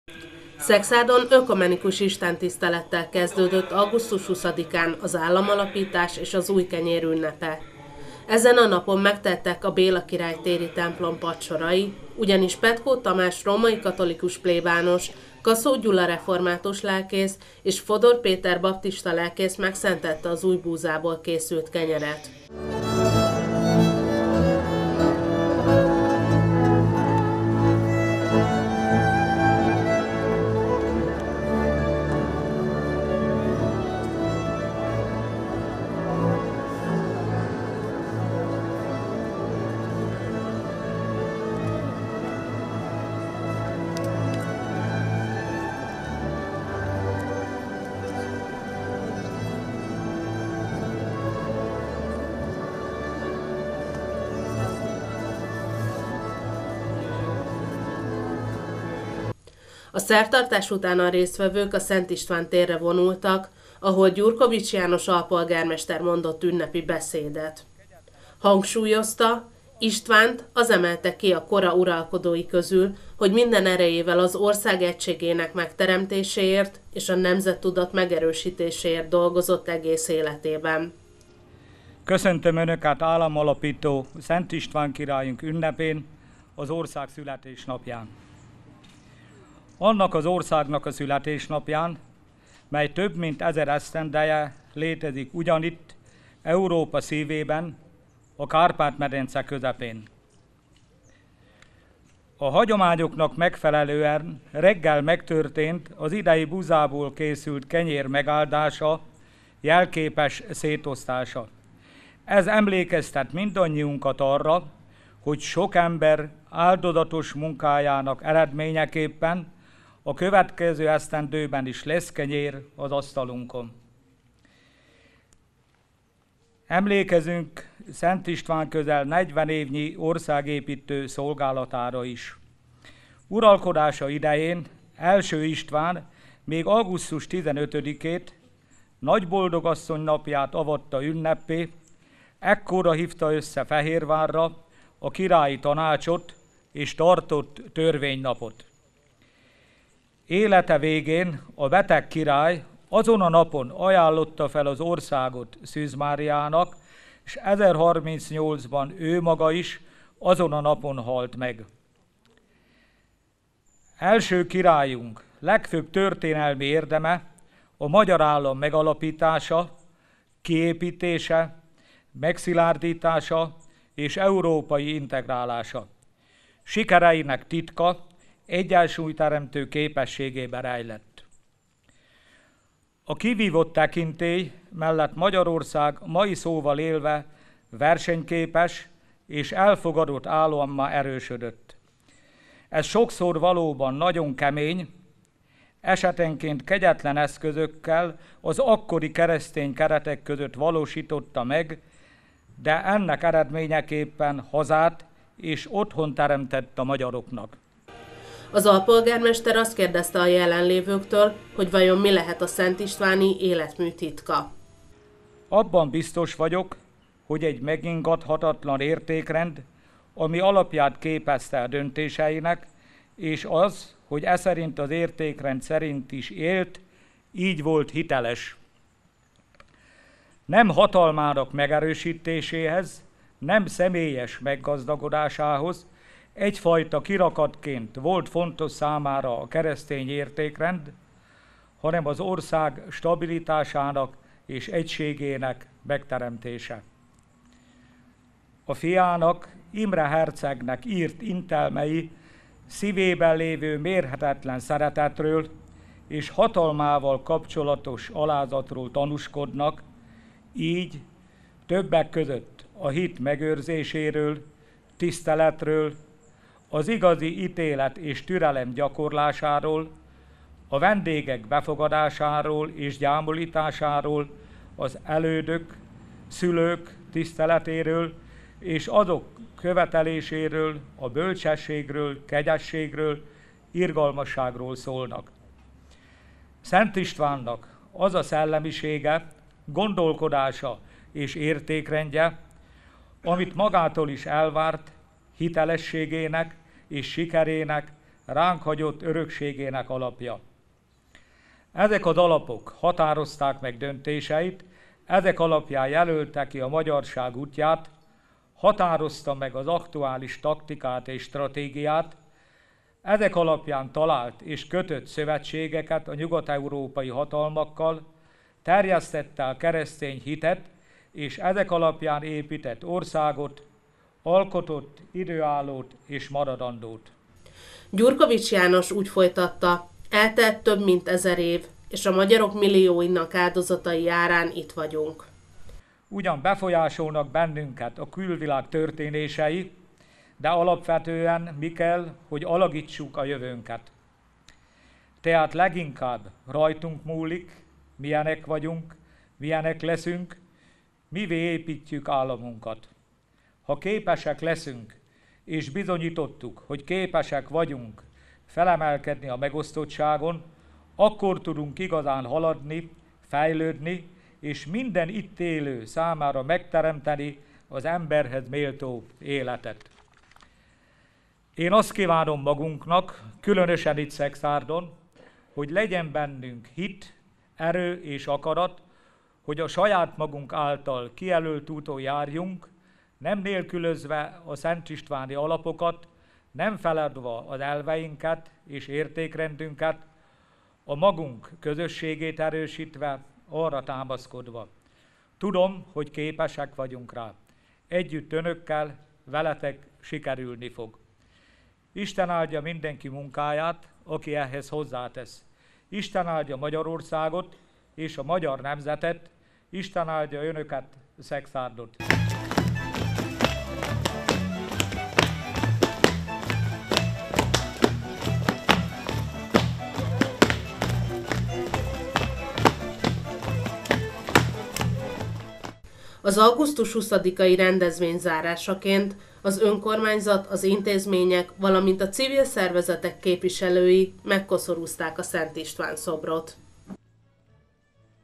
Szekszárdon is megünnepelték az államalapítás évfordulóját
Tolna vármegye szinte valamennyi településén, így Szekszárdon is megünnepelték az államalapítás évfordulóját. Augusztus 20-án szentmisével, kenyérszenteléssel, és koszorúzással tisztelegtek Szent István király emléke előtt a tolnai vármegyeszékhelyen.